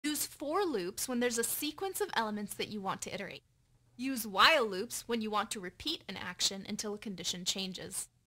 python | while loops vs for loops
When to use while loops vs for loops?
In some situations, you can use either while or for, then it's up to you.